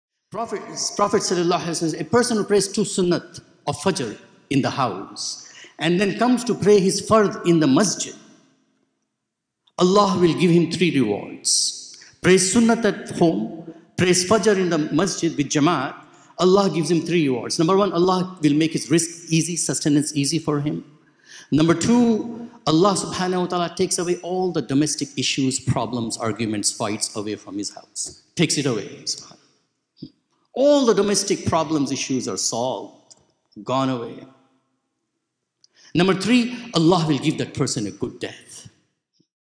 The Three Rewards for a Person Who Prays Fajr at the Mosque | Mohammed Abdul Baseer
Mohammed Abdul Baseer gives comforting spiritual insights about the three amazing benefits of praying the early morning prayer at the masjid after praying the Sunnah prayer at home: his rizk (daily sustenance) will becomes easy for him, his domestic issues will disappear, and that person will have a good death.

- More Br. Mohammed Abdul Baseer

This sermon was delivered at the Muslim Community Center - East Bay (MCC East Bay) in Pleasanton, California on August 5, 2022.